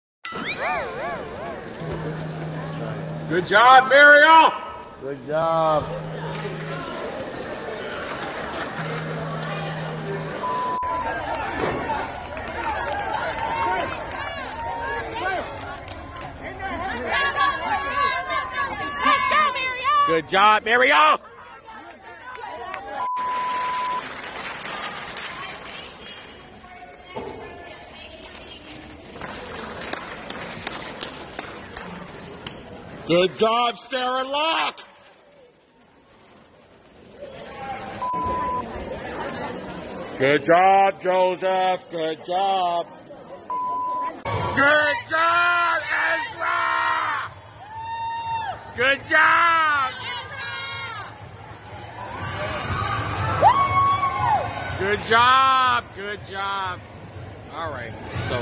I Know you hear me
A compilation of videos where a father shouts out to his kids "Good job" very loudly. Filmed in Suffolk, VA.